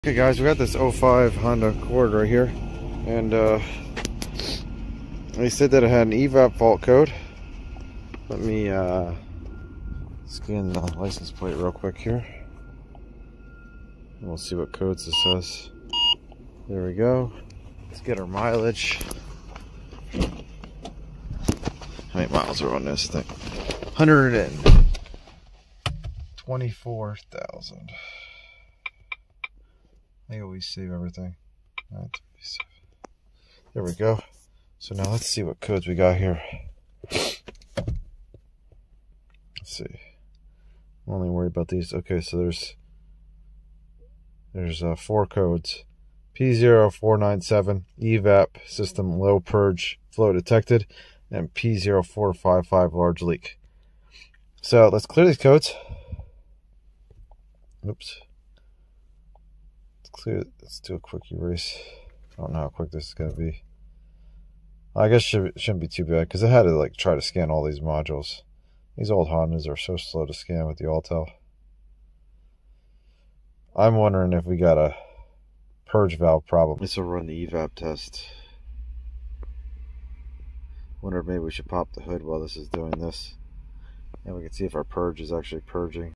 The EVAP Ghost Strikes: P0455 & P0497 on a 2005 Honda Accord! Can they nail the leak?
A classic case of automotive mystery unfolds as two tricky EVAP codes, P0455 (Large Leak) and P0497 (Low Purge Flow), appear on this older Honda Accord. Observe the meticulous process begin: scanning the modules with the Autel tool, clearing the faults, and immediately diving into command execution of the EVAP system test routine. The real detective work kicks in when the focus shifts squarely onto the purge valve assembly. Discover the step-by-step checks, from observing purge action during the test to applying vacuum gauges and load testing the circuit with a simple test light. It’s a masterclass in isolating a system notorious for hiding its faults.